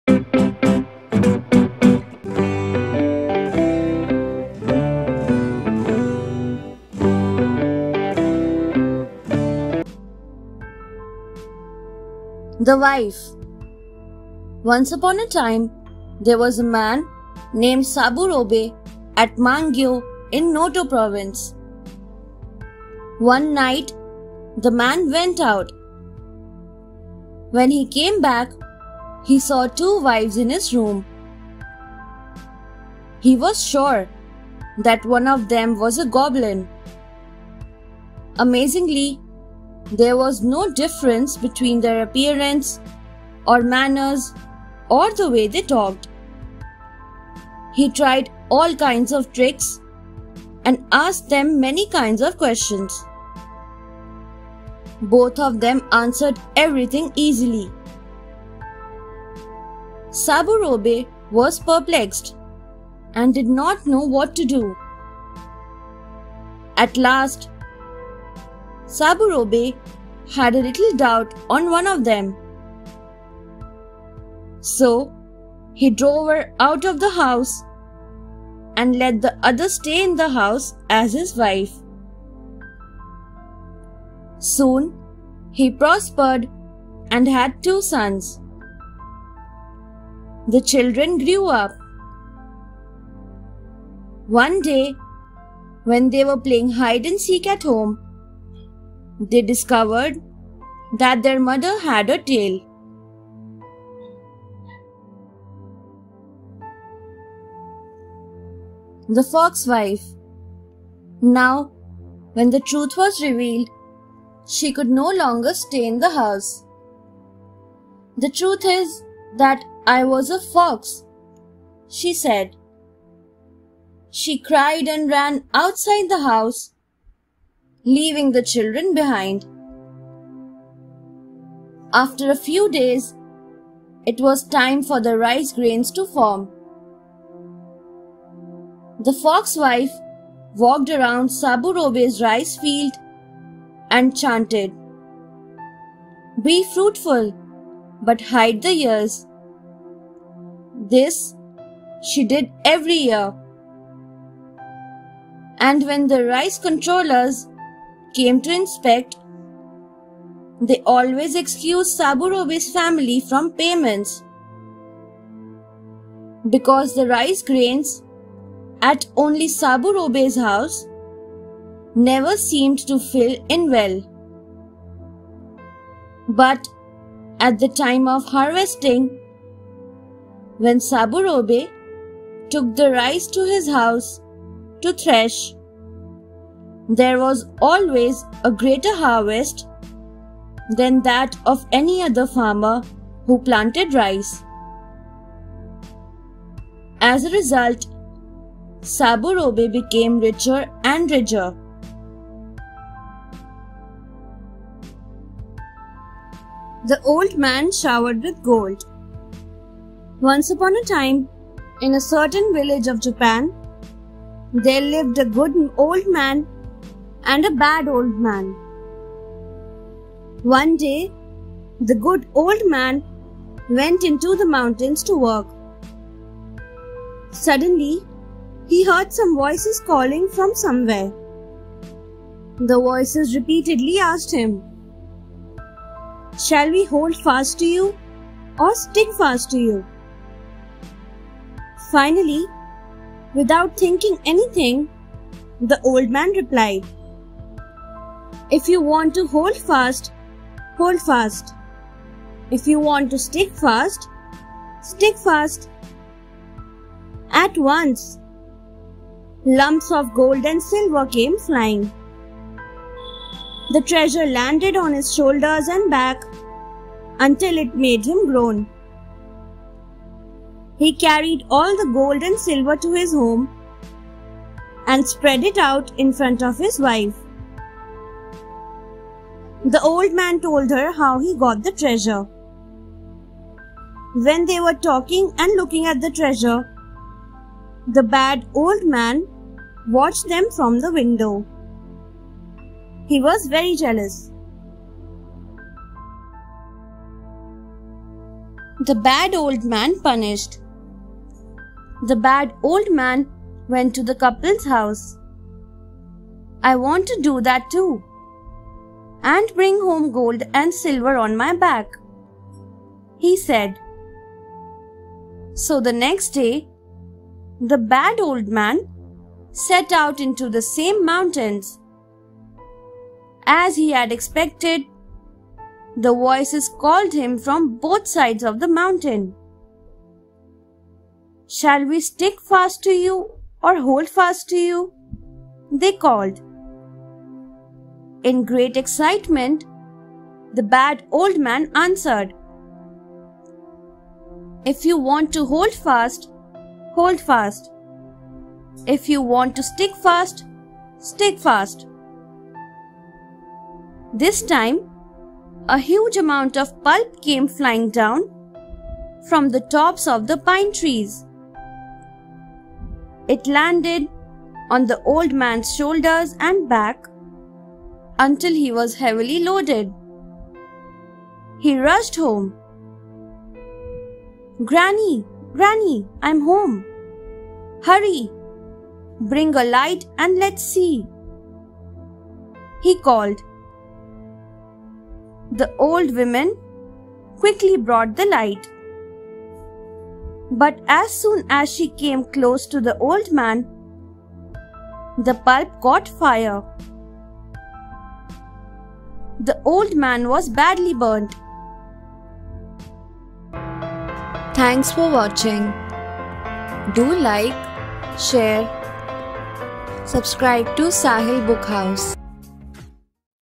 Fun Tales from Japan - Short Stories for Kids in English
Introducing Short Stories for Kids in English “Fun Tales from Japan World” which you and your Kids will love watching.
151 Tales from Japan World Stories contains Short stories with Pictures that children find interesting easy to learn and relate to, which helps them to develop confidence when they face real-life problems.